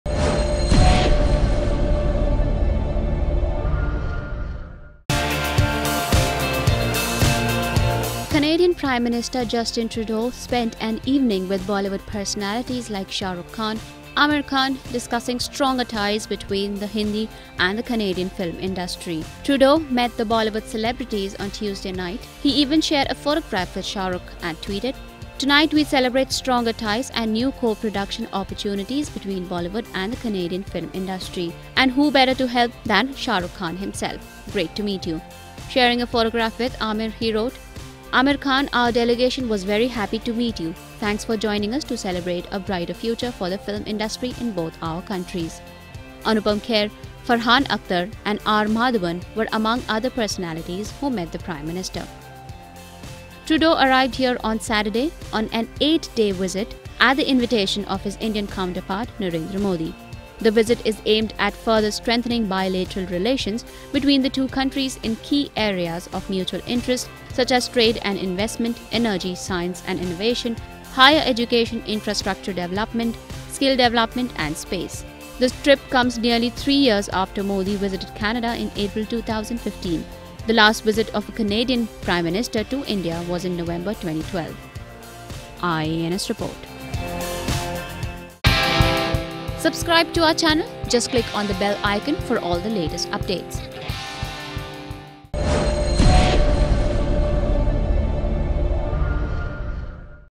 Shah Rukh, Aamir meet Canadian PM Justin Trudeau
Canadian Prime Minister Justin Trudeau spent an evening with Bollywood personalities like Shah Rukh Khan, Aamir Khan discussing stronger ties between the Hindi and the Canadian film industries. The trip comes nearly three years after Modi visited Canada in April 2015. The last visit of a Canadian Prime Minister to India was in November 2012.